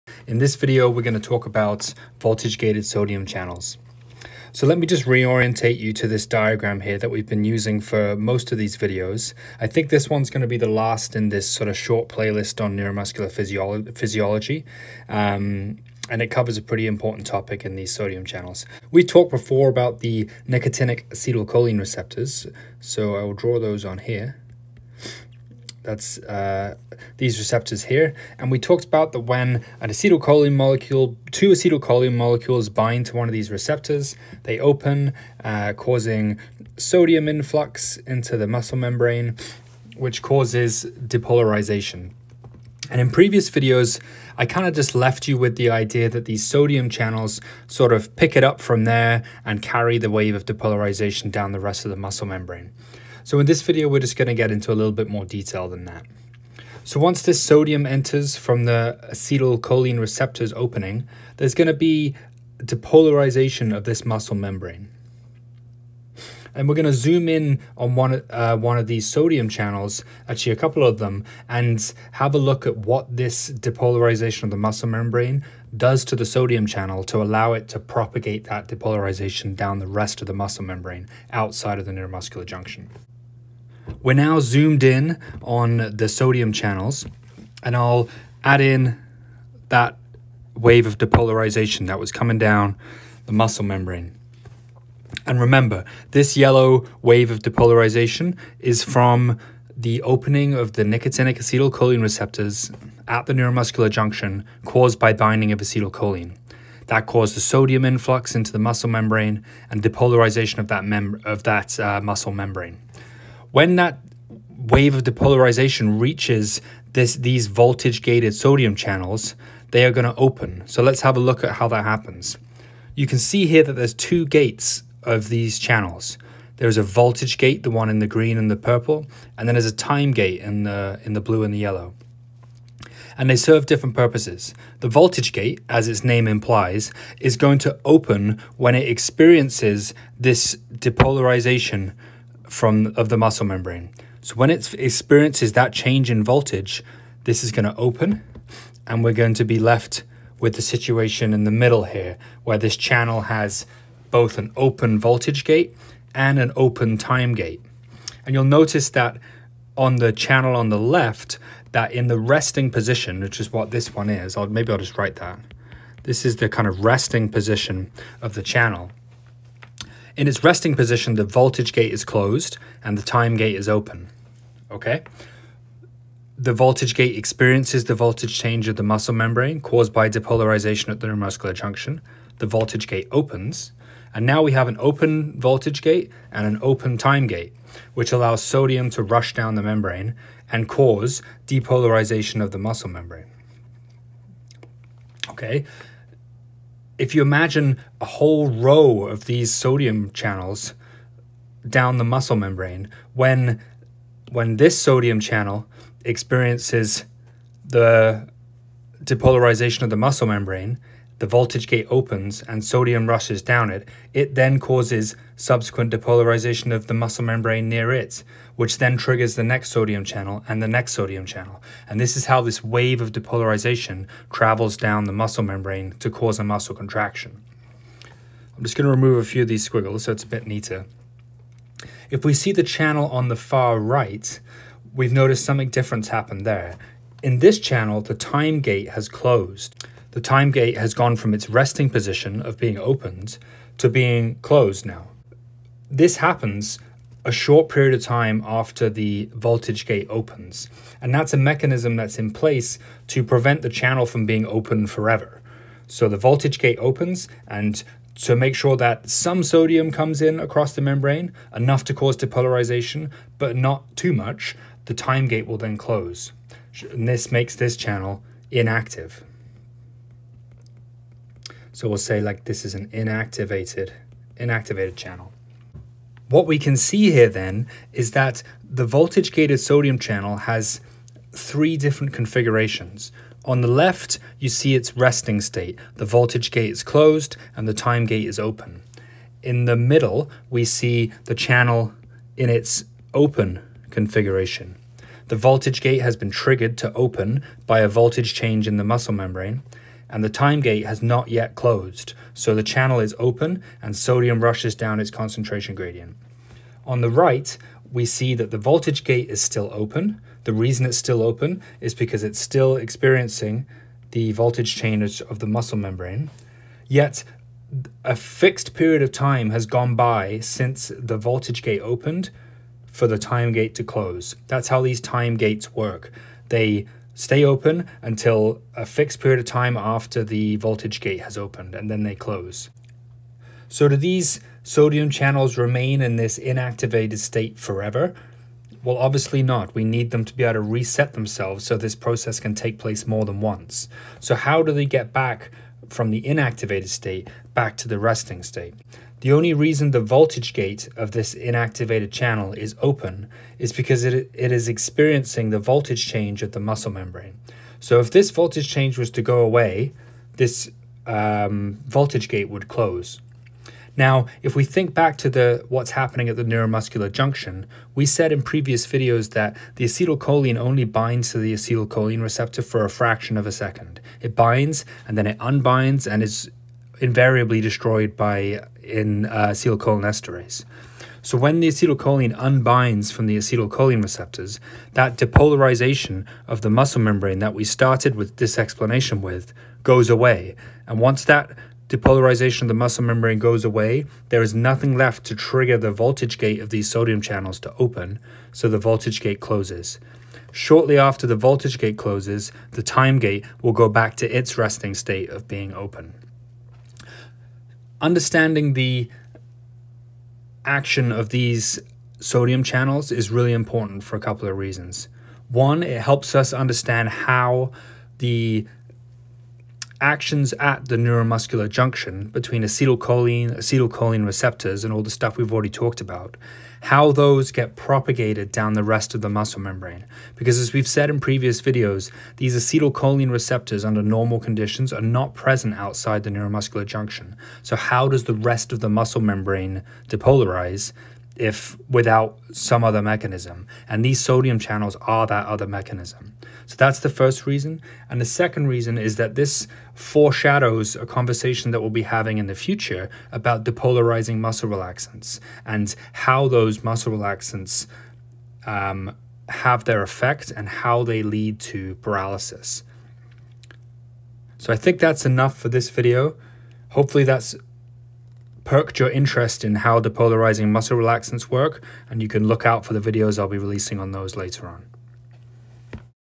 NM physiology 7: Voltage gated sodium channels
This is the last video in a series on neuromuscular physiology, targeted at anesthesia and critical care providers.

The major reference for this video (and the NM physiology series) is Miller’s Anesthesia 9th edition: chapter 12 Neuromuscular physiology and pharmacology. Miller is one of the major reference texts for anesthesiology and I highly recommend it to any of you interested in delving deeper into these topics.

Special thanks to Dr. Ben Schelew for his peer review of this video (and all others in this series on NM physiology/pharmacology). Dr Schelew is an attending anesthesiologist at the QEII Health Sciences Centre in Halifax NS, and has interests in thoracic anesthesia and medical education.